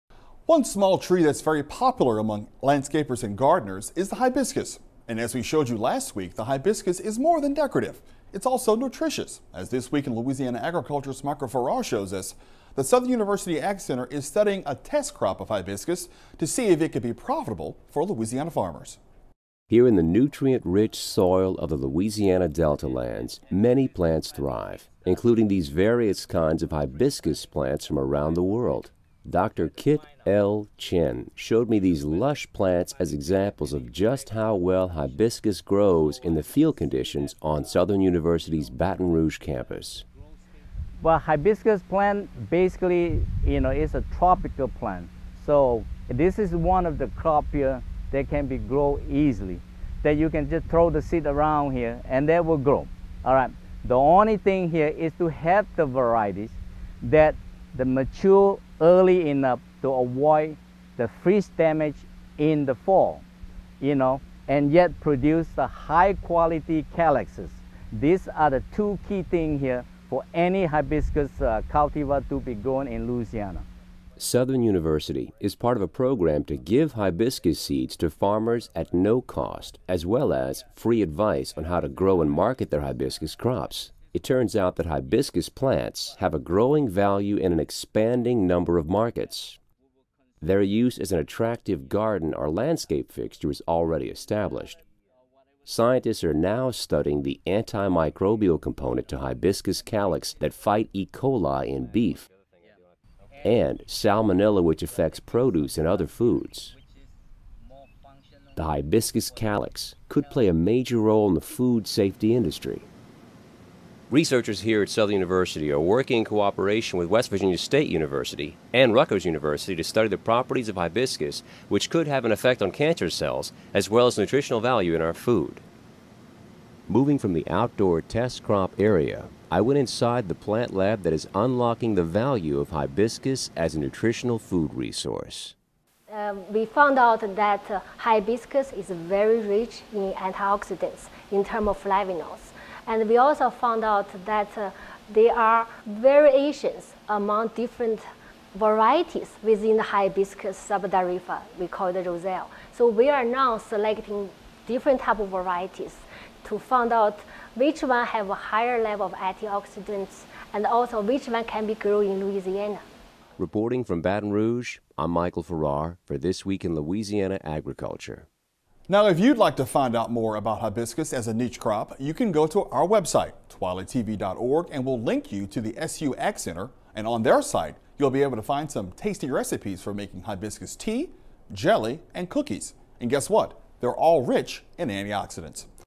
HIBISCUS NUTRITION
TWILA TV's Michael Farrar tells us how the Southern University Ag Center is studying a test crop of Hibiscus to see if it would prove profitable for Louisiana farmers.